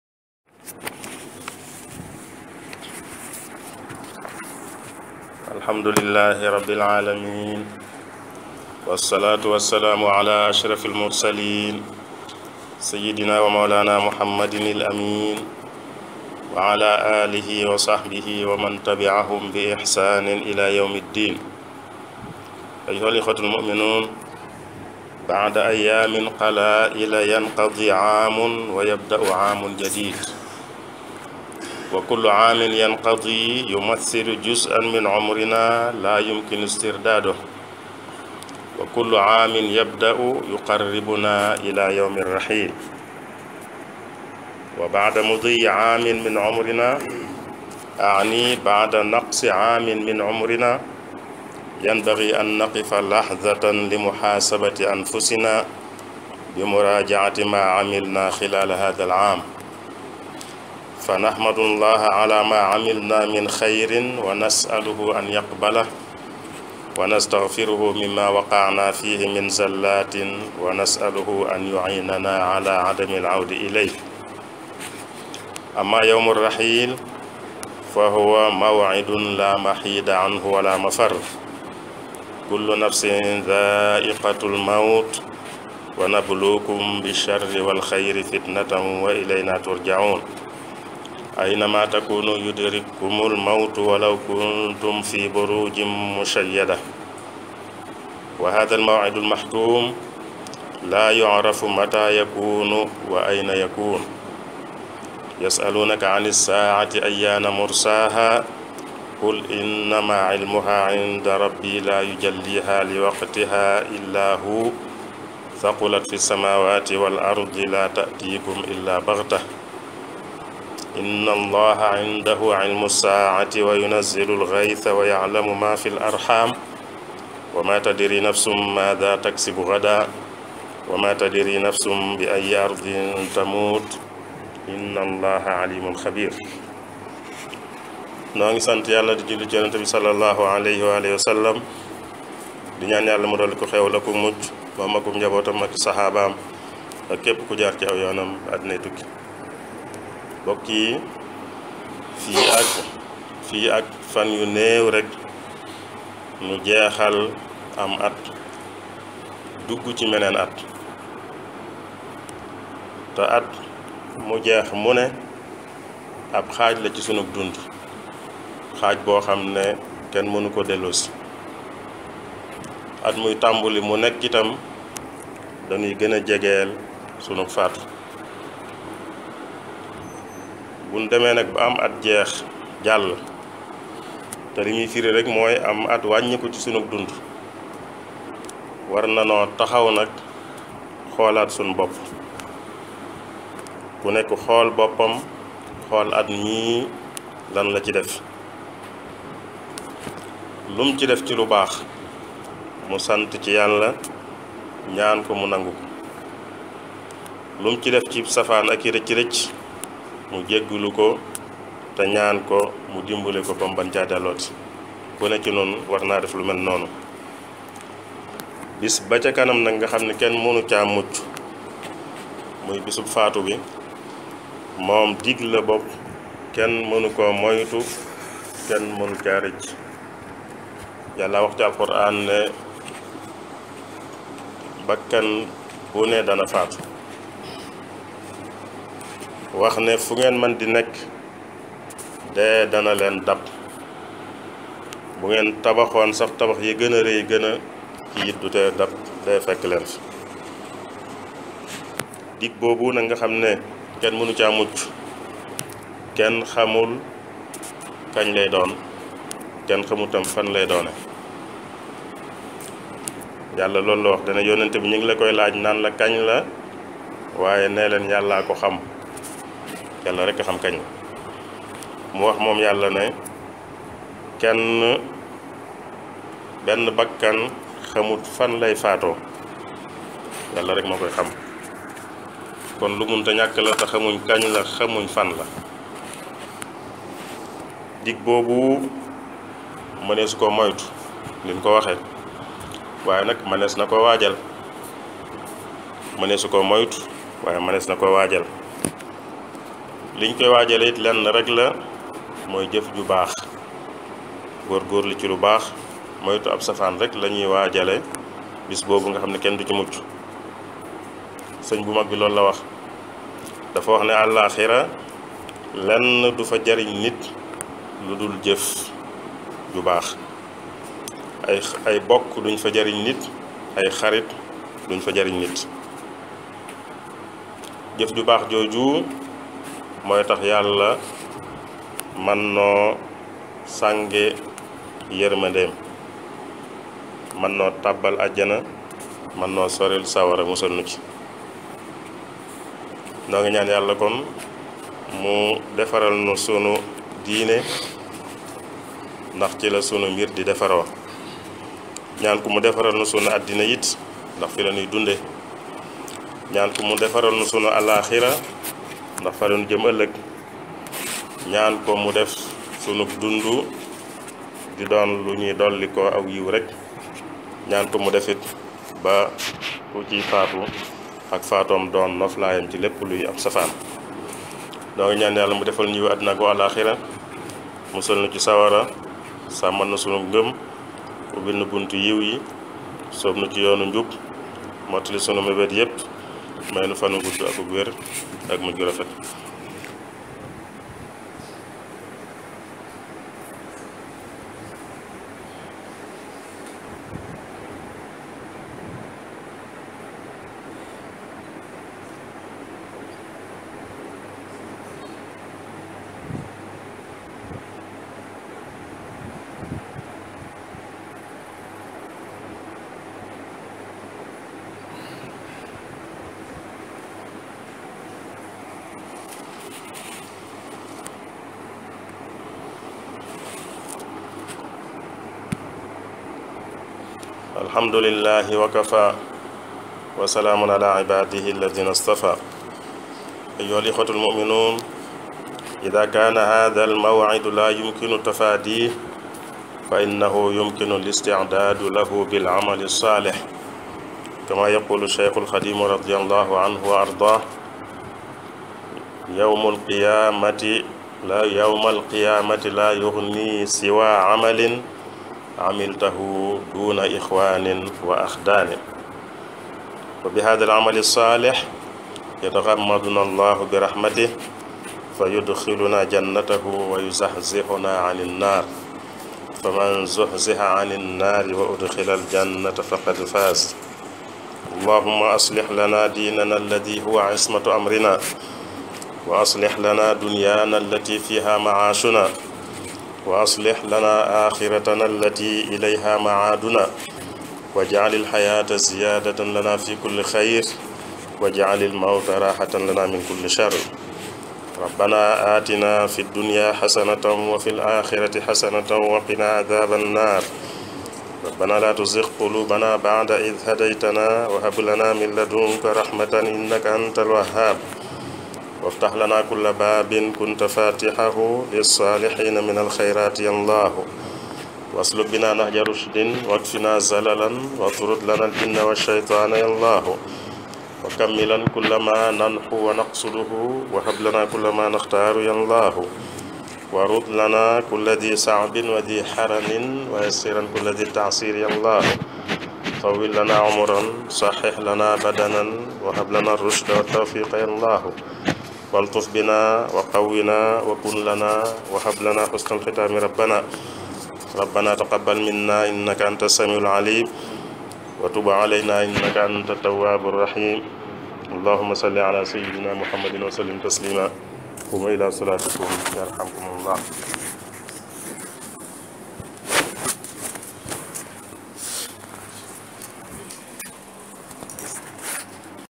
خطبتي رقم 248 وقفة مع النفس في نهاية العام 26 12 39 07 09 18
خطبة شَيخنا امباكّي عبد الودود في مسجد العارفين بحي بستان العارفين في طوبى، يوم الجمعة 26 من ذي الحجة 1439هـ (07 من سبتمبر 2018مـ)